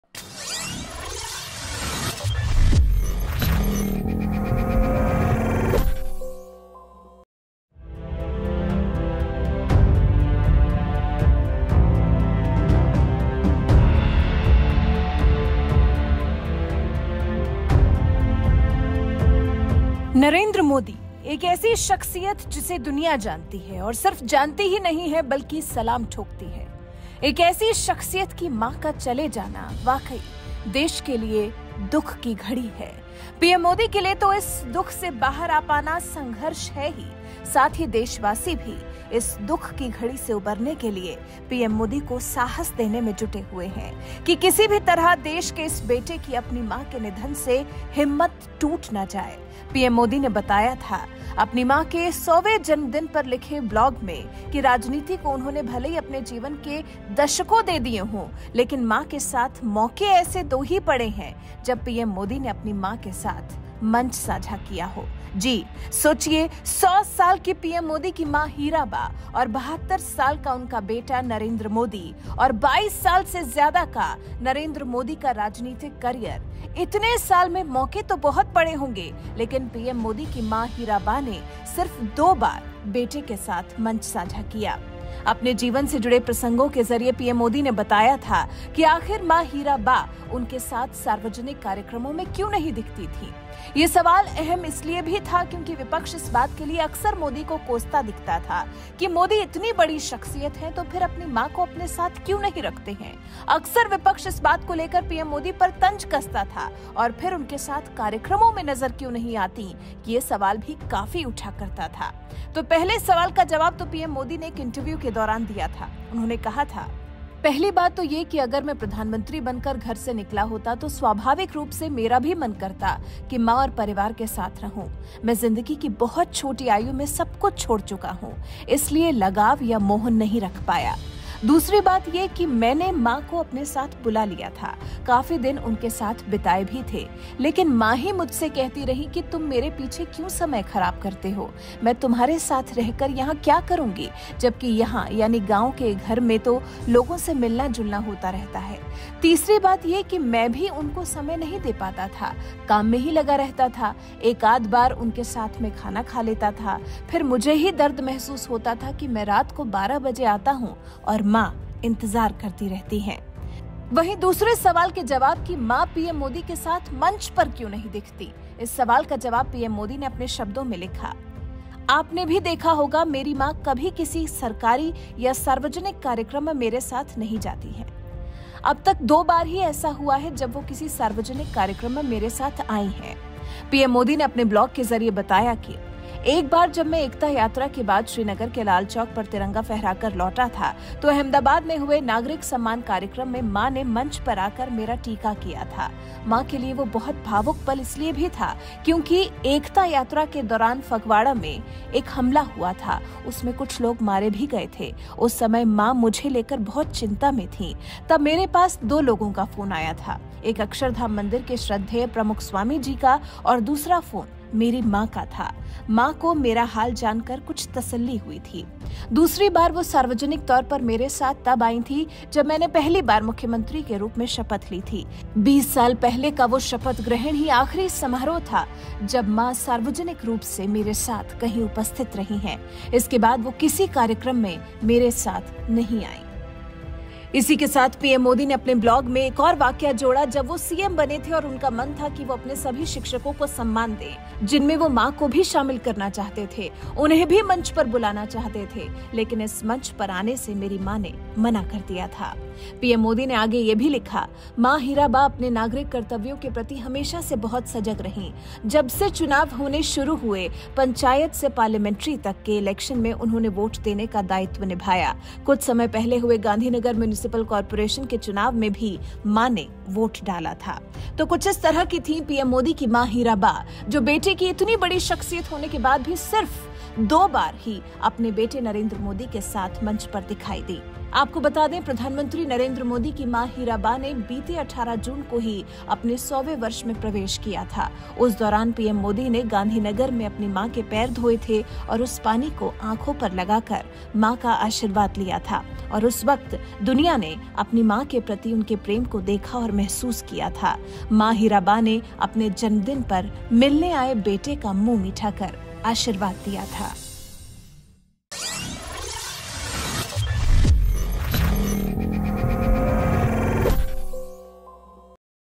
बेटा देश का प्रधानमंत्री और मां सिर्फ़ दो बार ही मंच पर दिखीं, PM Modi ने बताई पूरी कहानी!
प्रधानमंत्री नरेंद्र मोदी की माँ ने बेटे के साथ सिर्फ़ दो ही बार मंच साझा किया है…आख़िर क्यों??? पीएम मोदी से जानिए।

vinay
nivedita